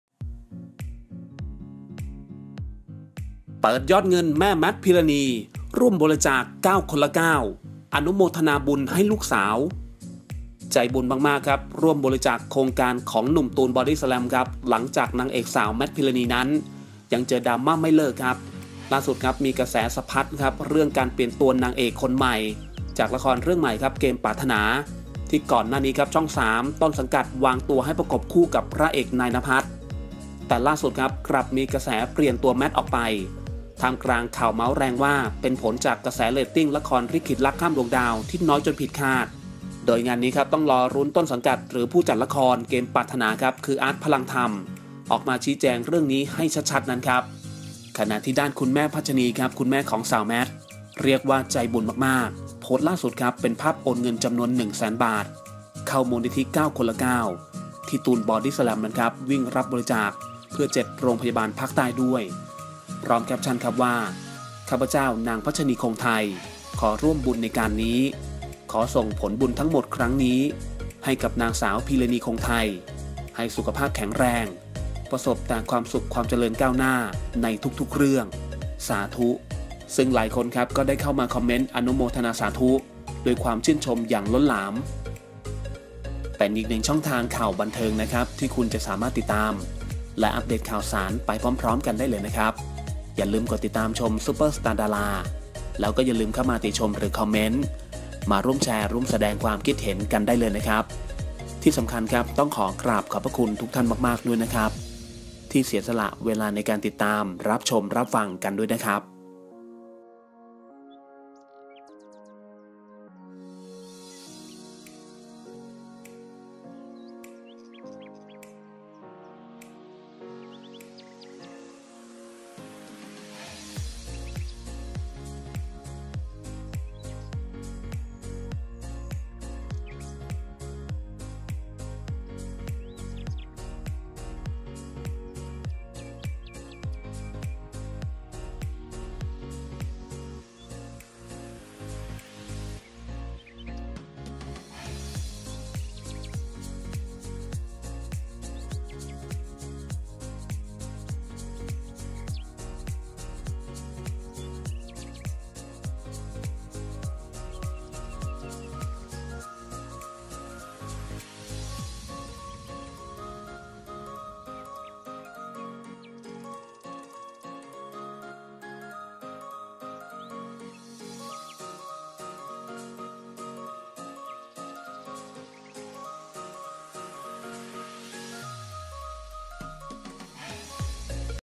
เปิดยอดเงิน แม่แมท ภีรนีย์ ร่วมบริจาค ก้าวคนละก้าว อนุโมทนาบุญให้ลูกสาว#super star dara
เครดิต:ขอขอบคุณภาพ :nee_phat

เปิดยอดเงิน แม่แมท ภีรนีย์ ร่วมบริจาค ก้าวคนละก้าว อนุโมทนาบุญให้ลูกสาว
ใจบุญมากๆร่วมบริจาคโครงการตูน – หลังนางเอกสาว แมท ภีรนีย์ ยังเจอดราม่าไม่เลิก ล่าสุดมีกระแสสะพัดเรื่องถูกเปลี่ยนตัวนางเอก จากละครเรื่องใหม่